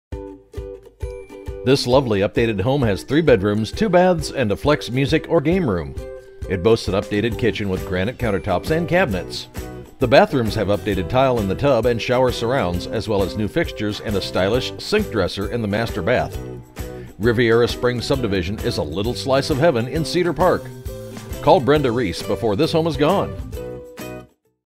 2403 Dior Dr. Cedar Park, TX
2403 Dior Dr., Cedar Park, TX

 This lovely updated home is three-bedroom, two-baths with an extra flexible music room. It boasts an updated kitchen with granite counter tops and cabinets. The bathrooms have updated tile in tub and shower surrounds, as well as new fixtures and stylish sink dresser in master bath. Riviera Springs subdivision is a little slice of heaven in Cedar Park.